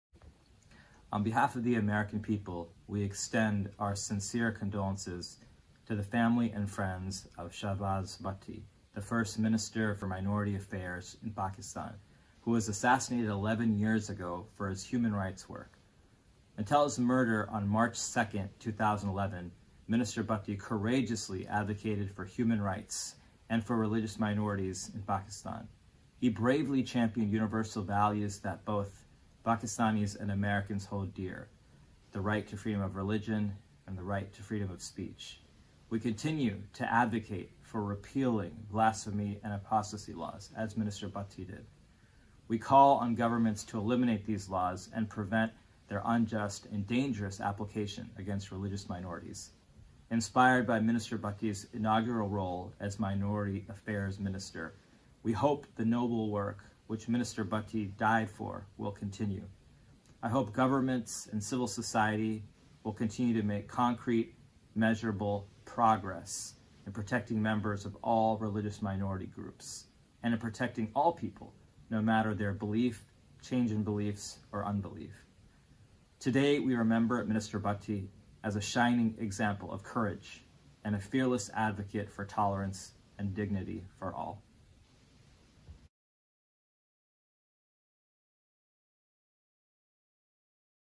Remembering Shahbaz Bhatti: Rashad Hussain, Ambassador for International Religious Freedom USA
'They can silence the man but not his vision'

Hear from Rashad Hussain, Ambassador for International Religious Freedom USAas we gathered to honour and remember Shahbaz Bhatti continuing his legacy in the struggle for religious freedom in Pakistan.